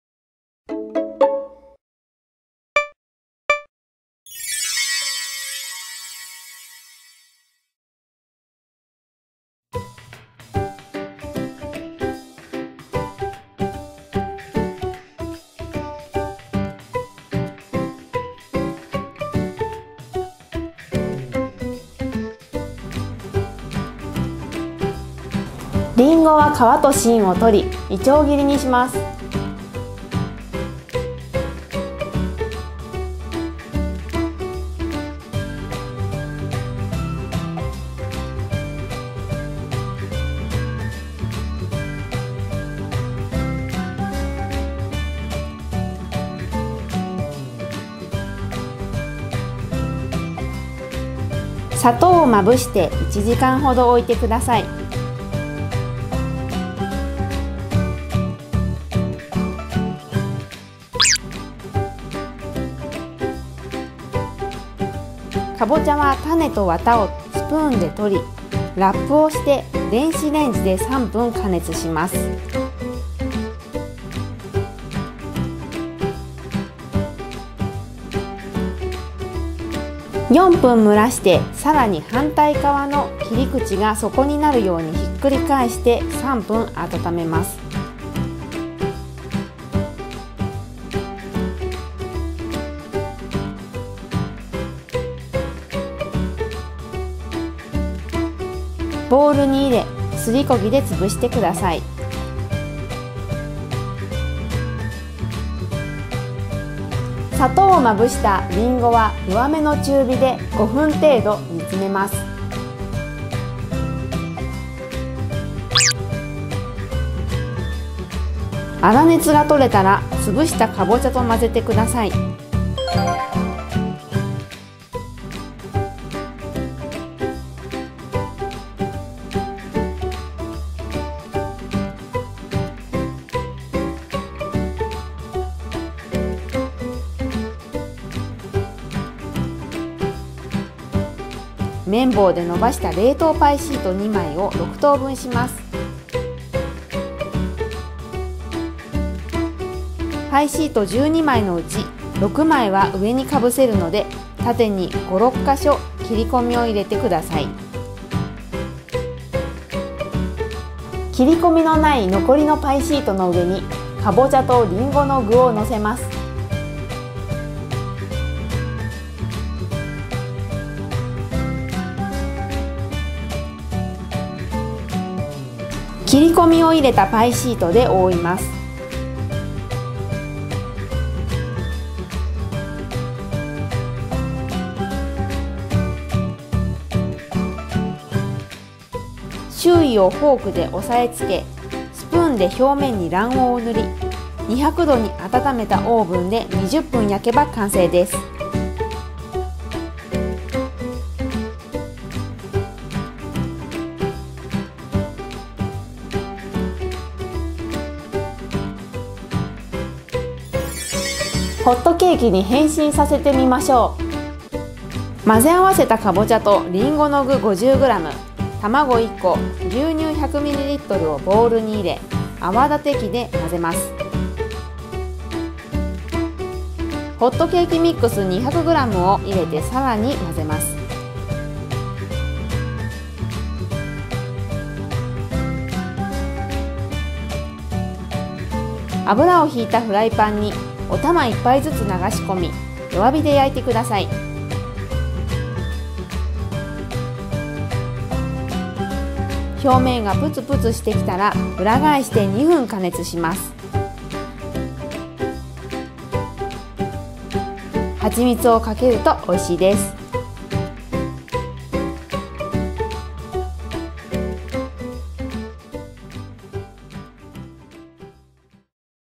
【レシピ】ハロウィンに作ろう！カボチャリンゴパイ
もうすぐハロウィーン。今年はどんな仮装をしようかな。おばけカボチャのランタンも用意して…。想像を膨らませていたら、先生がカボチャを使ったお菓子を提案してくれました。
【パーティー料理・関連動画】

【先生】
野菜ソムリエ　大橋千珠（おおはし・ちず）さん

★中日こどもウイークリーのお申し込みはこちら